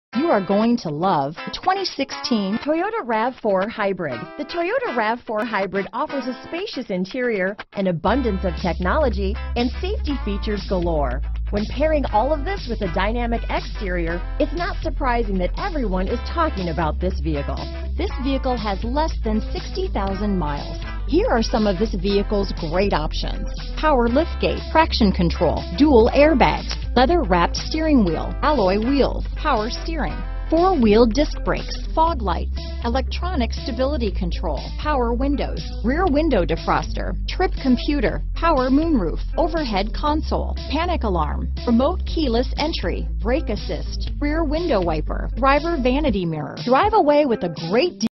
2016 Toyota RAV4_Hybrid Alpharetta, Roswell, Cumming, Sandy Springs, Marietta, GA 204618A
Super White Used 2016 Toyota RAV4_Hybrid available in Alpharetta, Georgia at Angela Krause Ford Lincoln. Servicing the Roswell, Cumming, Sandy Springs, Marietta, GA area.

Recent Arrival! THIS IS A REALLY NICE HARD TO FIND RAV4 HYBRID AWD. THIS HAS GREAT SERVICE HISTORY. THIS IS A HARD TO FIND SUV. THIS WONT BE HERE LONG. COME SEE THIS TODAY! 2.5L 4-Cylinder 6 Speakers Alloy wheels Exterior Parking Camera Rear Front dual zone A/C Power Liftgate Power moonroof Power windows Radio: Entune Audio Plus AM/FM/MP3/WMA Playback Remote keyless entry.brbrOdometer is 2105 miles below market average! Clean CARFAX.brbrSuper White 2016 Toyota RAV4 Hybrid XLE 2.5L 4-Cylinder AWD 34/31 City/Highway MPGbrbrAwards:br  * 2016 IIHS Top Safety Pick+